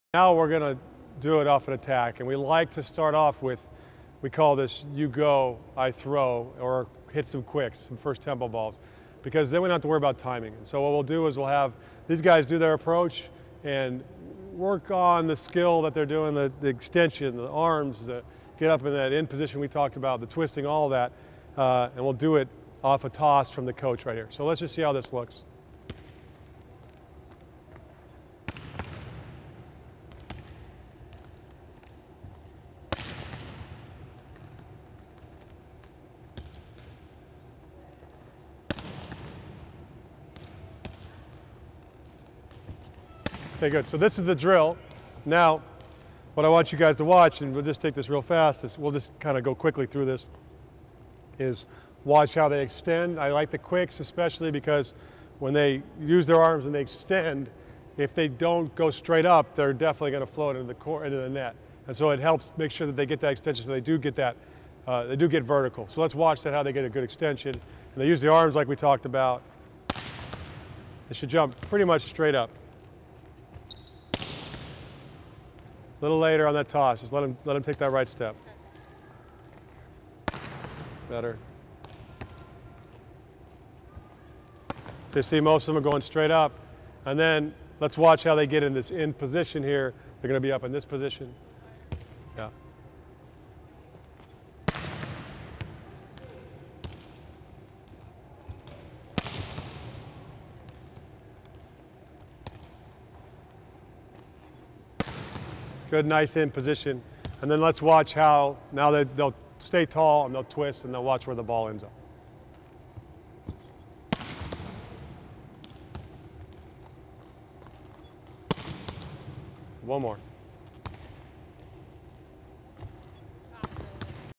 Develop Stronger Attacks by Improving Your Player's Movements!
Kevin Hambly is the University of Illinois Head Coach and his team was the 2011 NCAA Runners-up. Coach Hambly will help your attackers develop confidence in their movements by showing them how to rotate and jump properly.